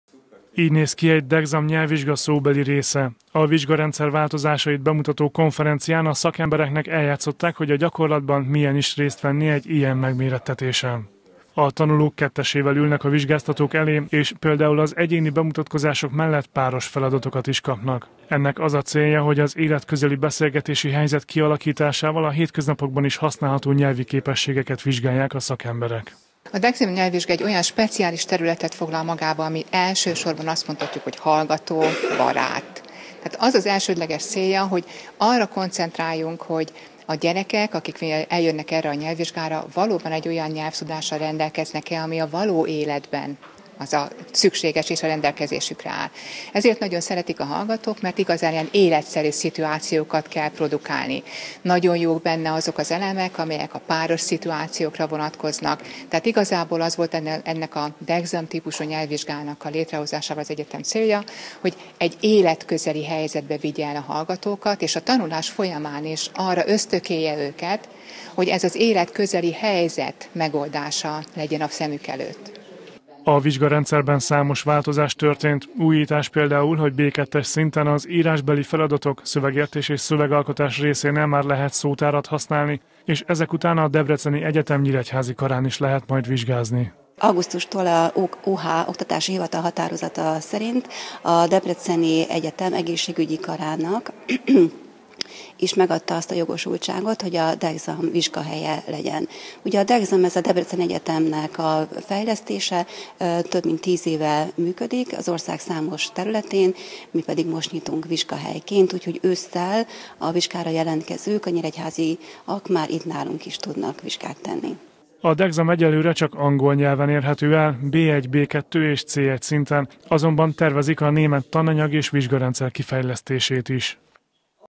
Nyíregyházán is elérhető a Dexam nyelvvizsga - Kölcsey Tv
A következő tanévtől már Nyíregyházán is elérhető a Dexam, amely a Debreceni Egyetem saját fejlesztésű nyelvvizsgarendszere.
Az idei évtől számos újítást vezettek be, ezen túl például használható szótár is az írásbeli vizsgák egyes részein, valamint a felsőoktatási intézmény Egészségügyi Karán is lehet majd bizonyítványt szerezni.